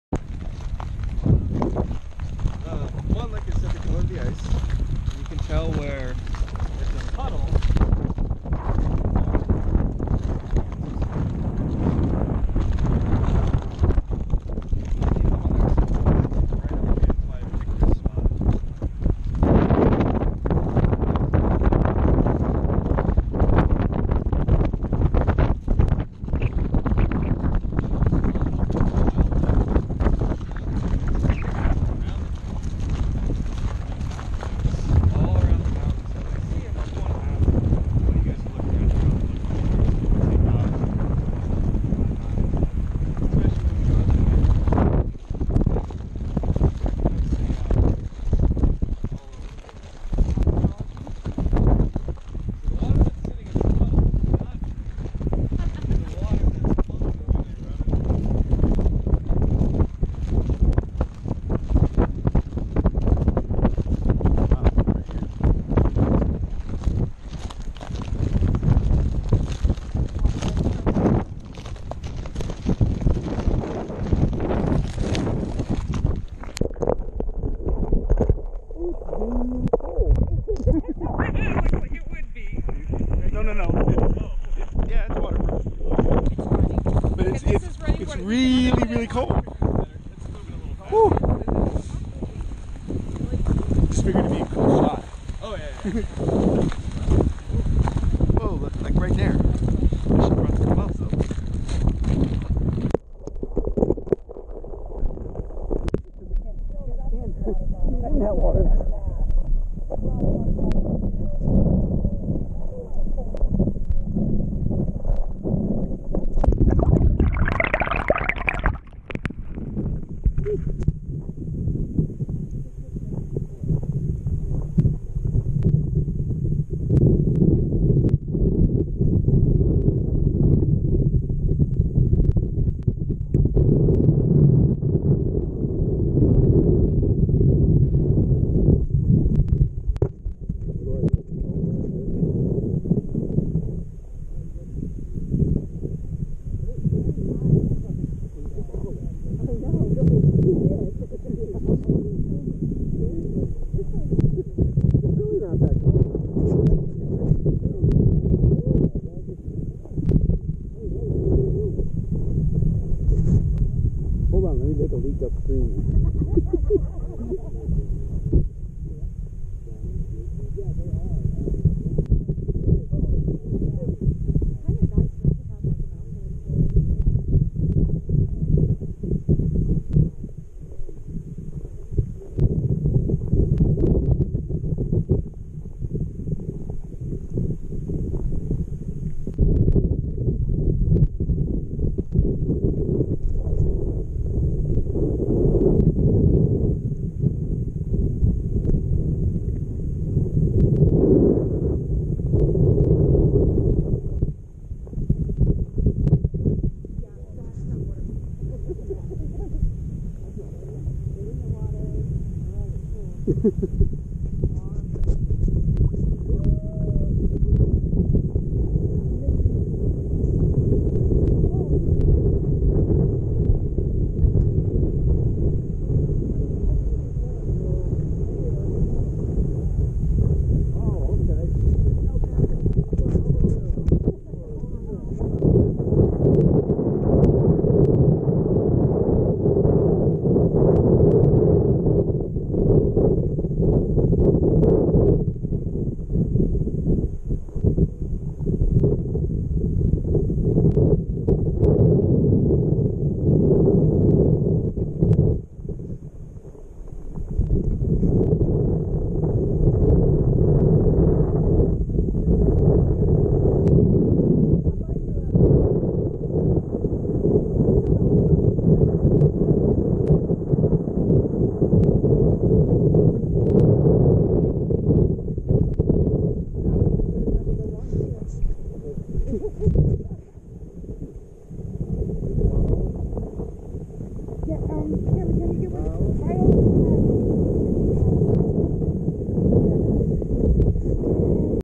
Ice cold and refreshing
Videos from Alaska, June 2016 Drinking some cool refreshing glacier water